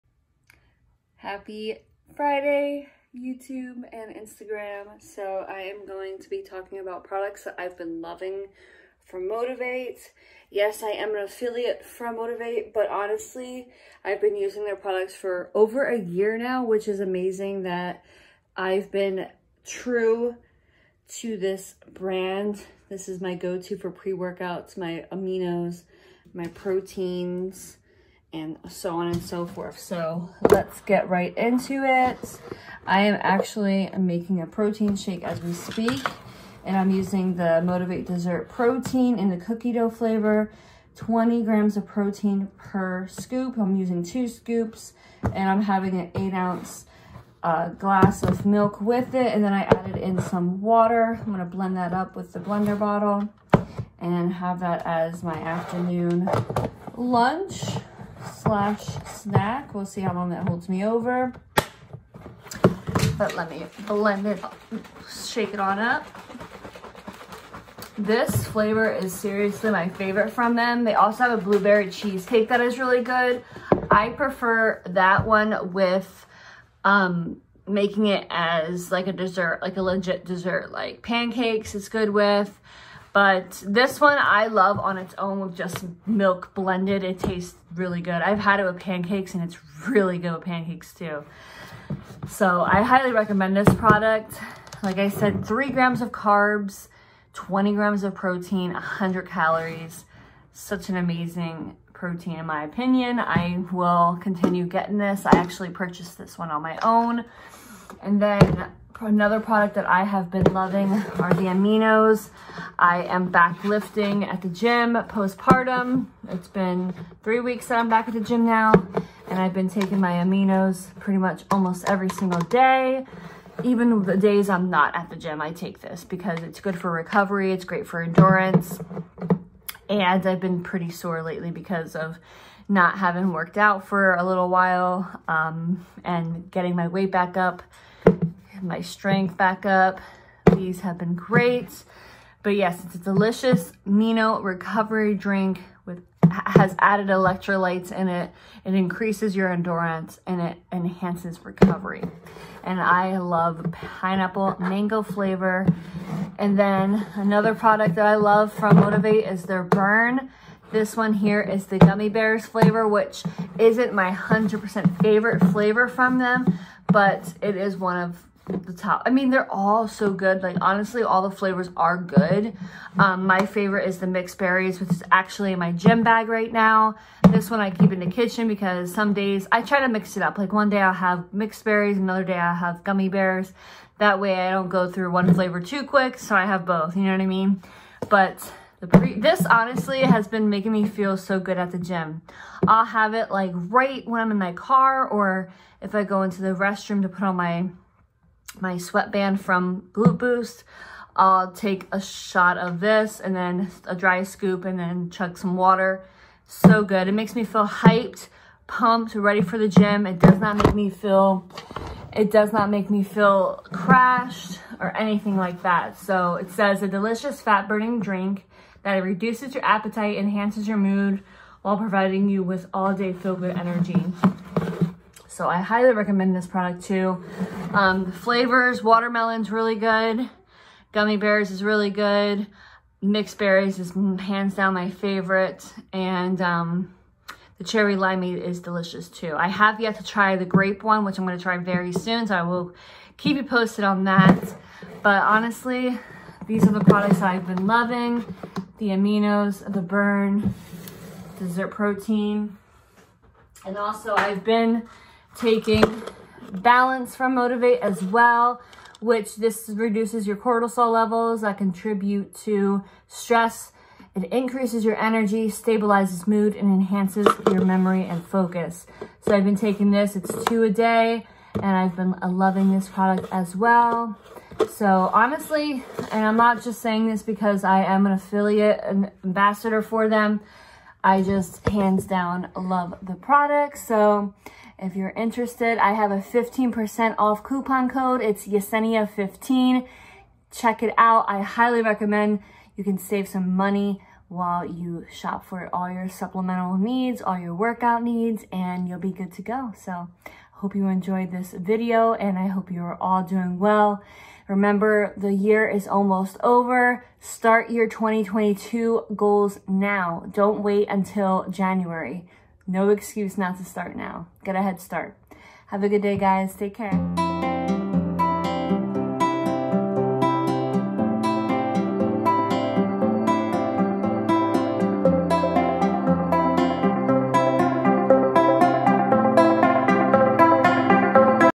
Motiv8 products I’ve been loving | Save 15% | Supplements | Yes_2Fitness
Today’s video I am talking about all the supplements and products that I have been loving￼￼ from Motiv8!!

My 6 week BODY CHANGER program is half off, NOW until end of year 12/31
DM on Instagram  if Interested 📧